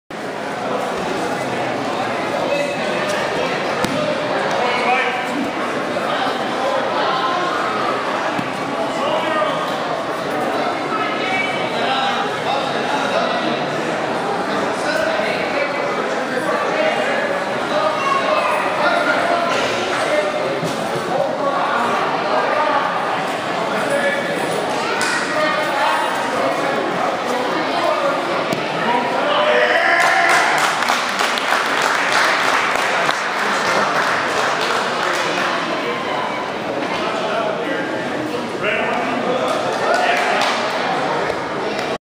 Pursely Popper at 2013 AAU Freestyle Judo Nationals (165lb division)
Derrick Darling throws his opponent at the Freestyle Judo National Championships and follows up with a Pursely Popper (collar choke). View #2.